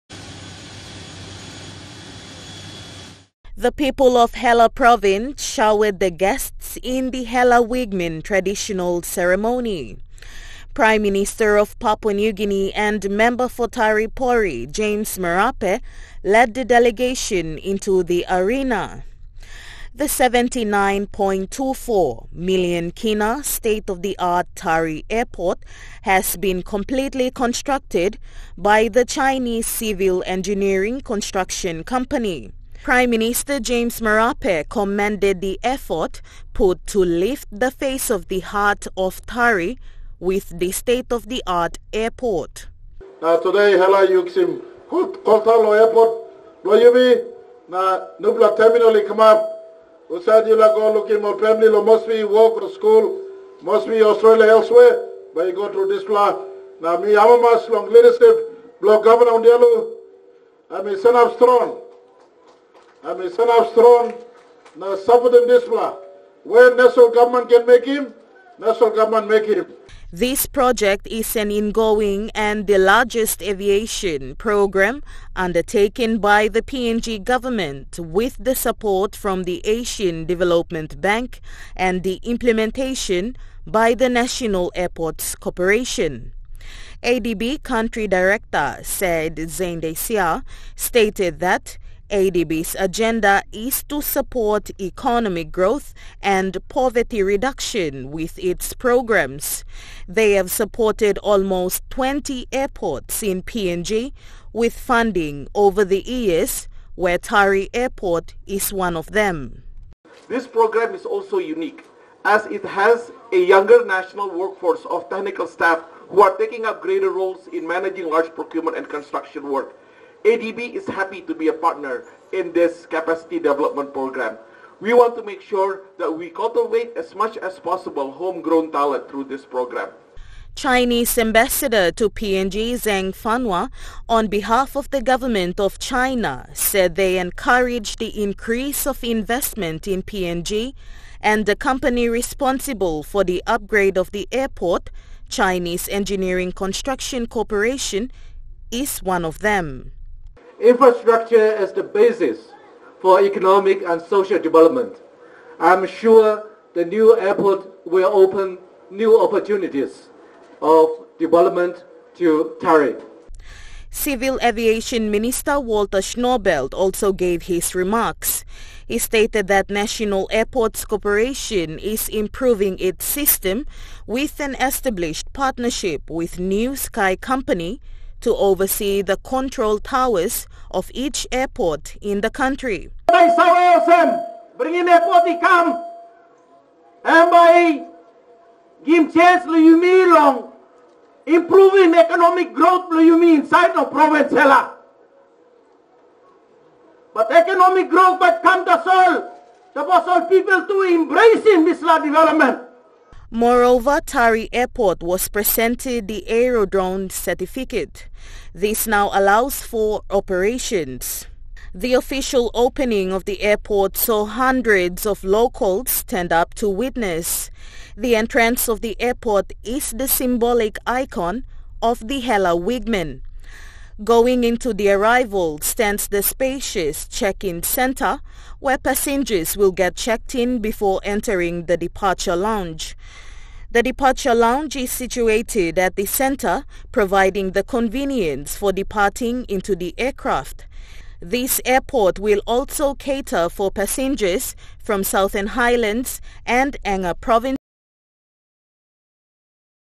NEW TARI AIRPORT
The official opening of the newly upgraded Tari airport project took place yesterday in Hela province.
Prime Minister James Marape with Minister for Civil Aviation and Transport Walter Schnaubelt and the National Airports Corporation made the formalities.
Present at the event was Prime Minister James Marape, Minister Walter Schnaubelt, Governor for Hela province Philip Undialu and other Ministers and members of parliament, Asian Development Bank country manager, Said Zaidansyah, Chinese Ambassador to PNG Zeng Fanhua and PNG LNG Managing Director, Managing Director for NAC and other key stakeholders.